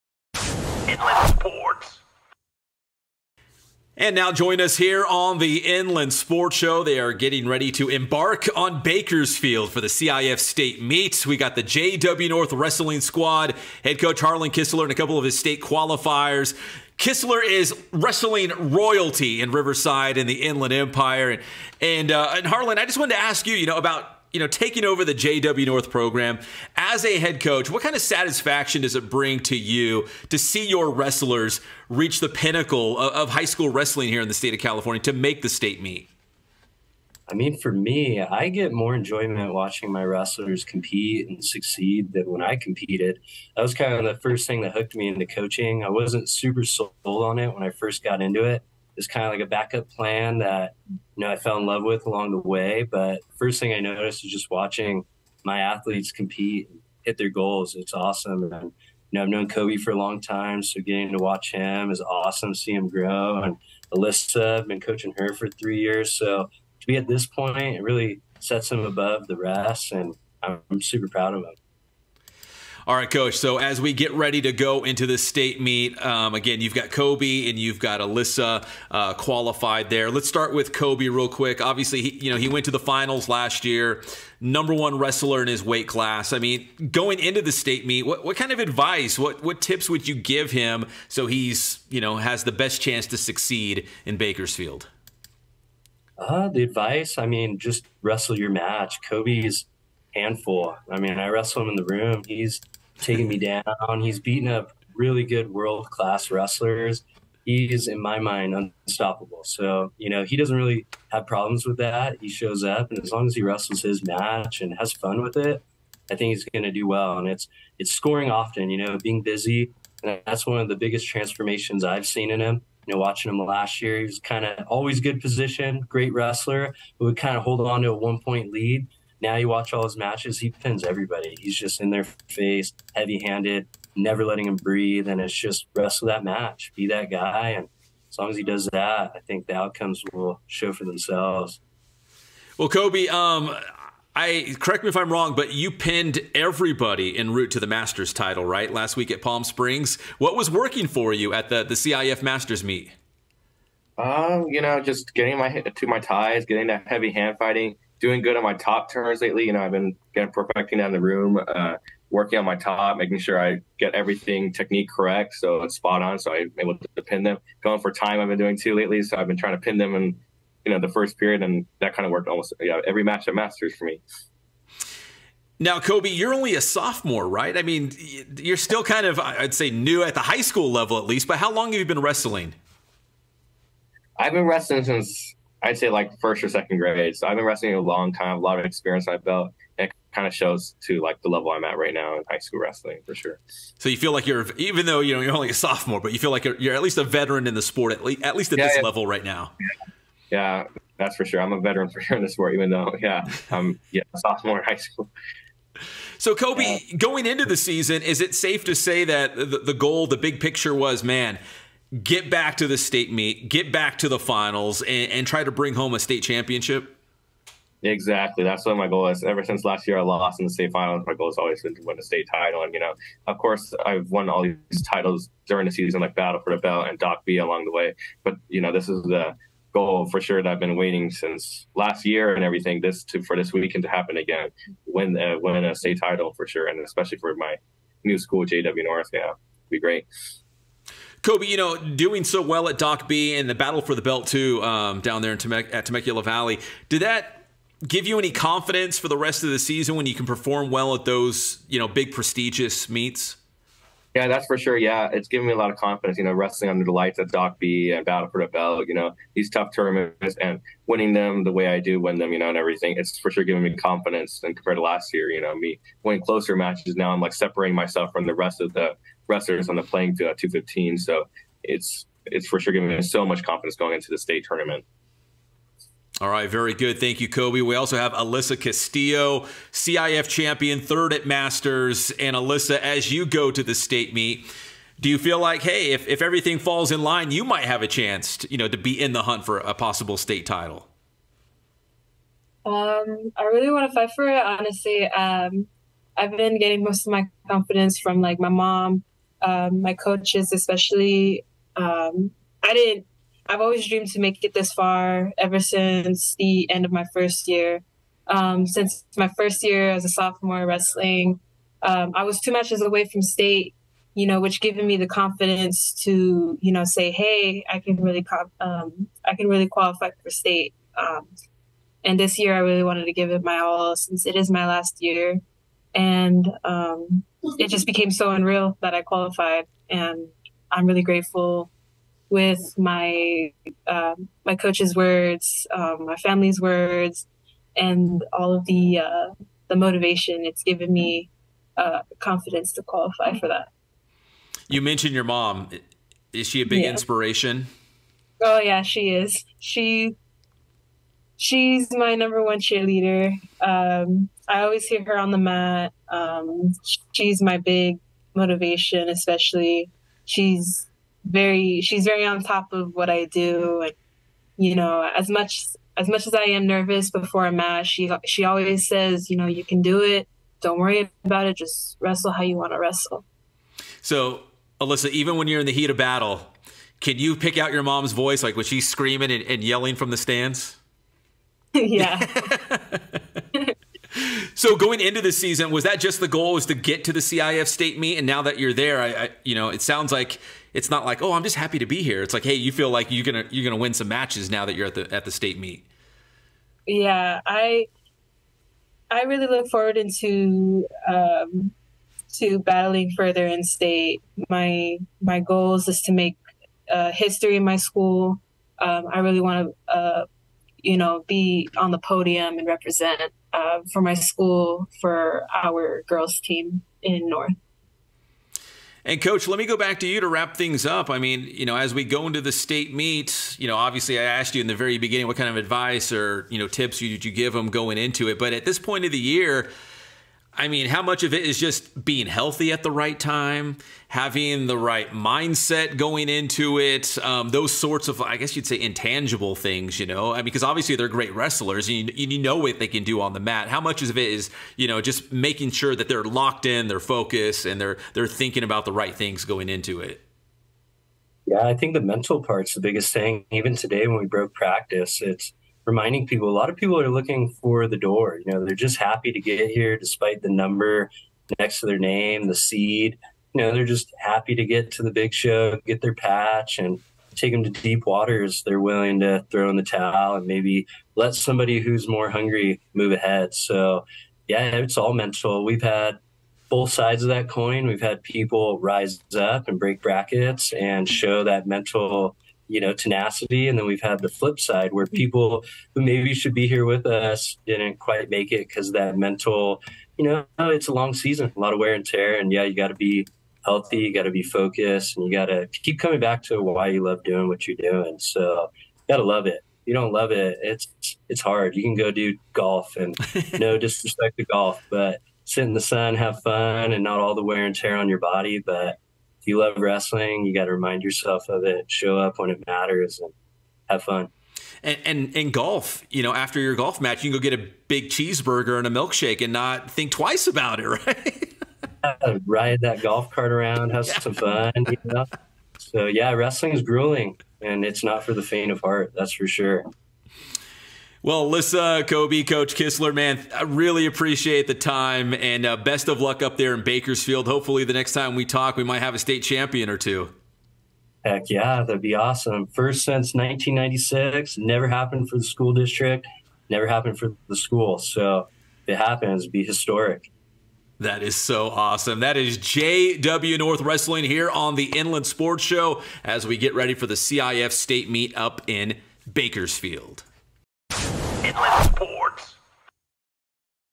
WRESTLING: JW North headed to CIF State Meet
Huskies head coach Harlan Kistler with Coby Merrill and Alissa Castillo.

Follow Inland_Sports, the local sports Leader in the InlandEmpire